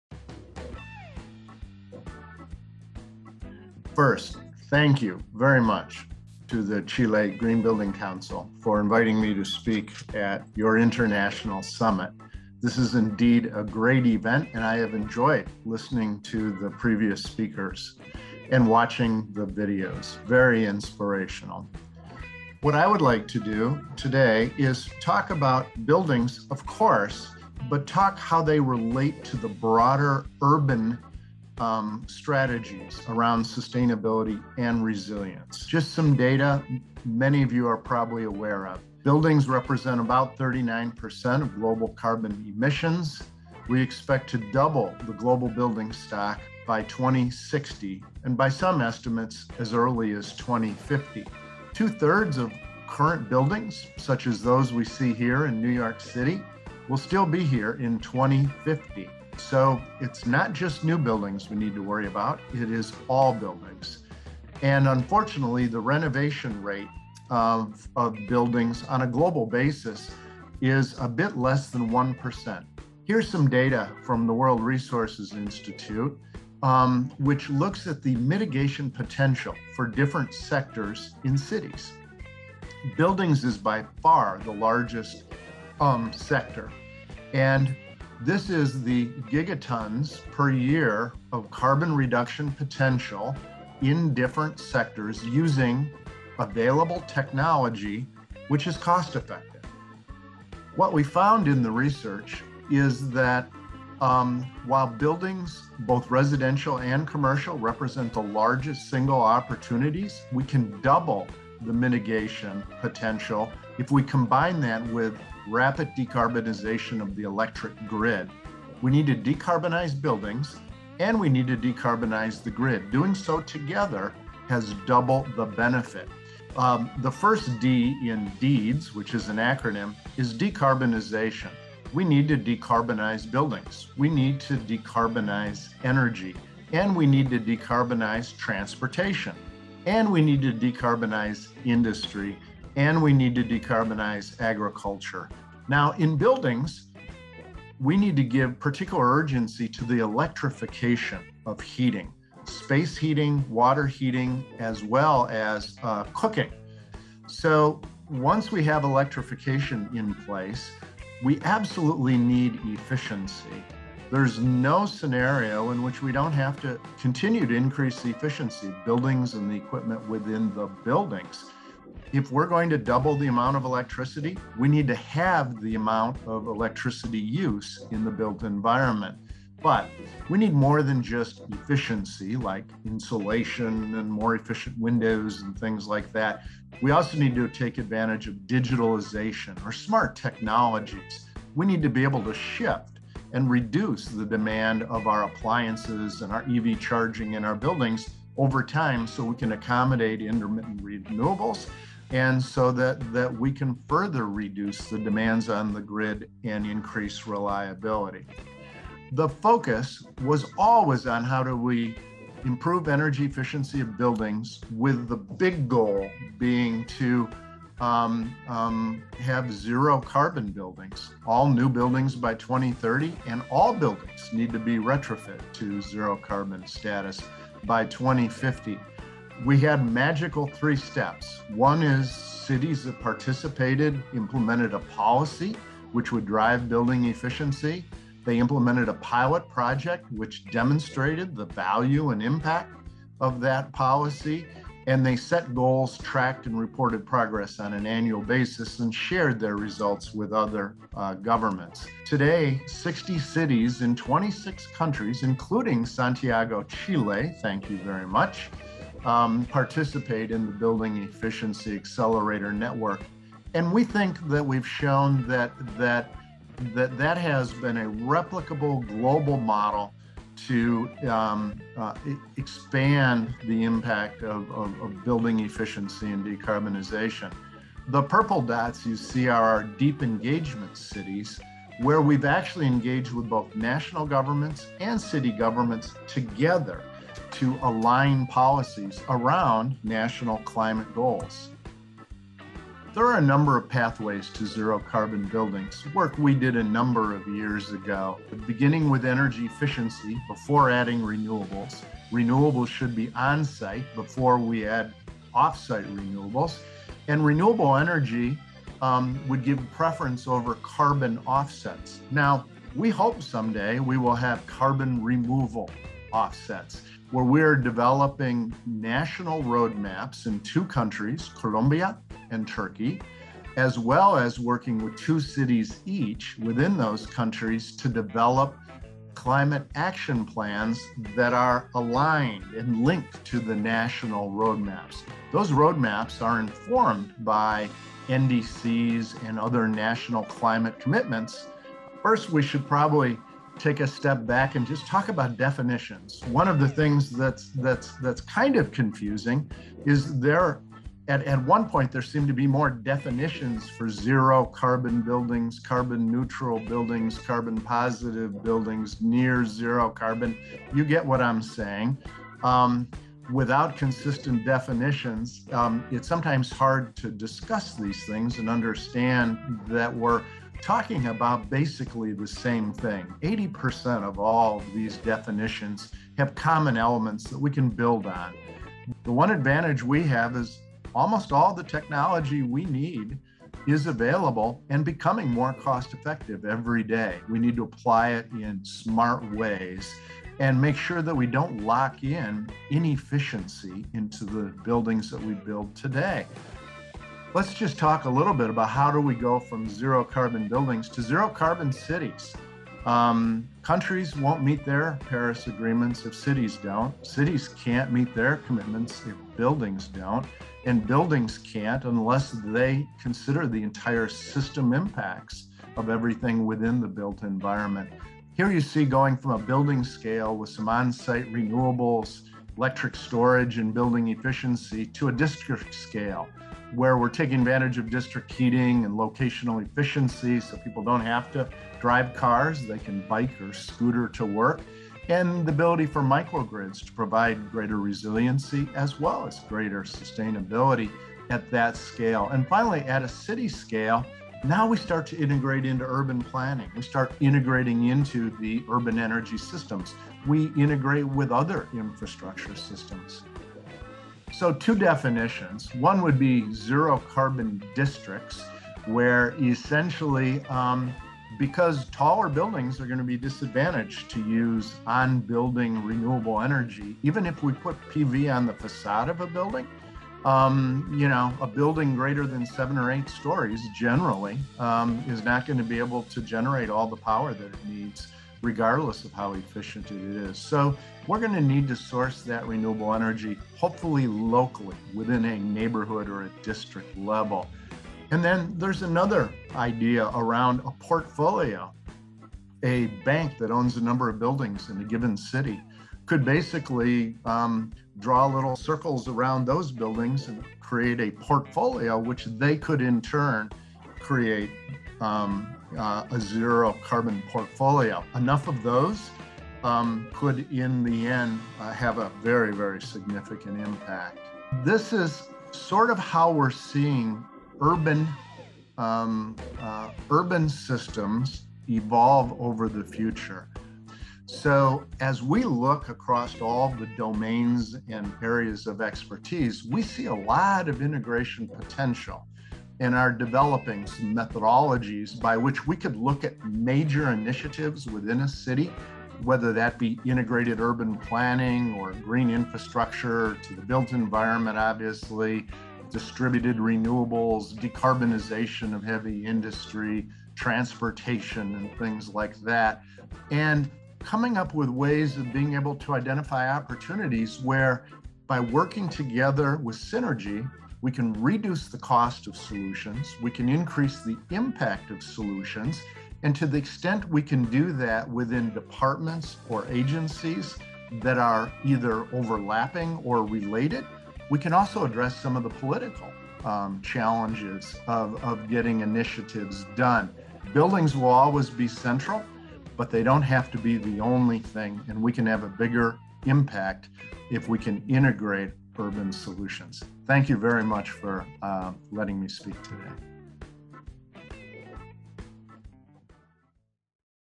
Clay Nesler, Global Lead, Buildings and Energy del World Resources Institute
Clay Nesler, Global Lead, Buildings and Energy del World Resources Institute Ross Center For Sustainable Cities expuso el tema “Integrated strategies for urban climate action and resilience", en el International Summit: Construcción Sustentable Resiliente al Cambio Climático realizado el 28 de octubre de 2021.